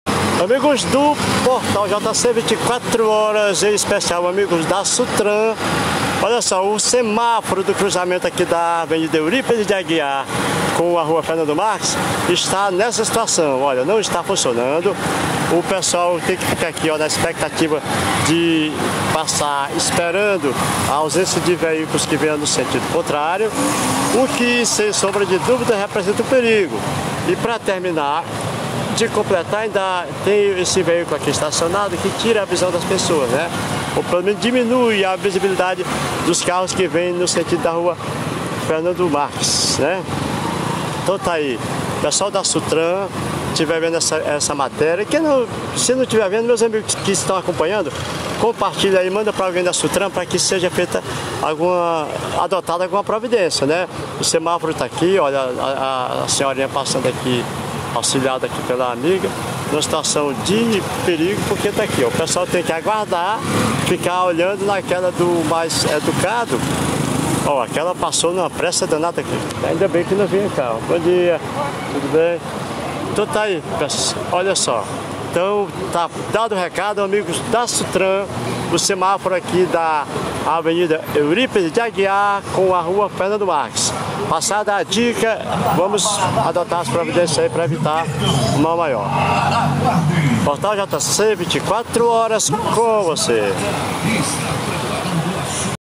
Semáforo do cruzamento da Rua Fernando Marques com Av Eurípedes de Aguiar deu pane
O semáforo instalado no cruzamento da Avenida Eurípedes de Aguiar com a Rua Fernando Marques, apresentou um defeito na manhã de hoje ficando totalmente fora de funcionamento. Através de uma chamada de vídeo, o Portal jc24horas alertou a equipe da Sutran para que adotassem as providências no sentido de resolver o problema e com isso, evitar um problema maior. Confira o vídeo.